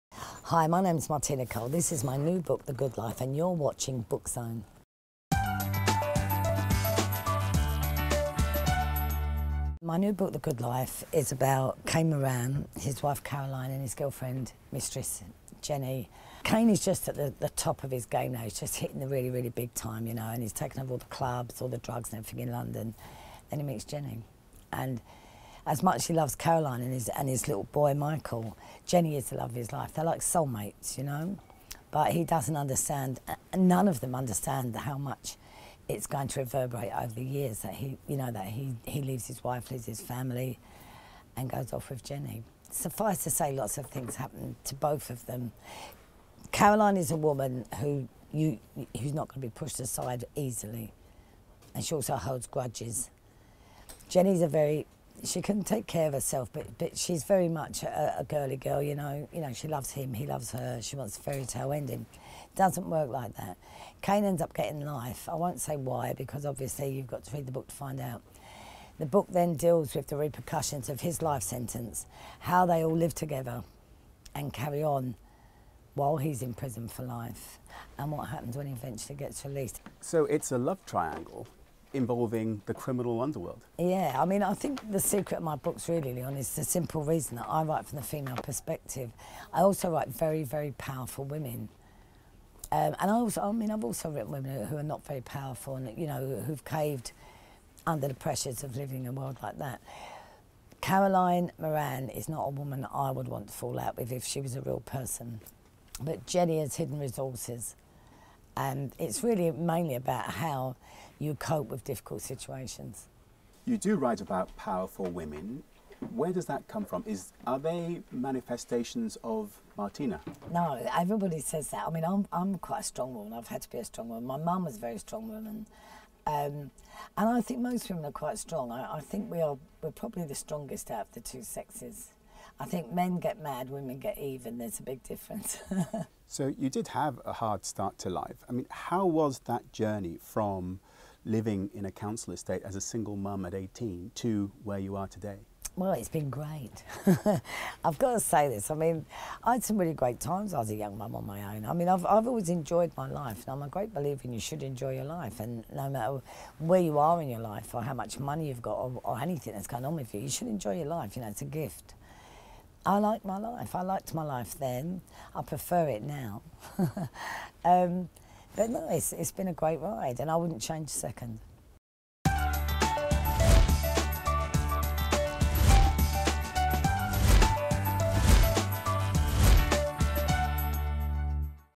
Martina Cole - 'The Good Life'
A love triangle set in London's criminal underworld… and Martina tells Book Zone about her own journey from council estate to bestselling author.